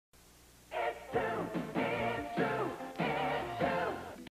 WCBS-TV | It's 2 - Station Bumper ID (1988)
1988 WCBS Channel 2 station ID from the slogan "It's 2".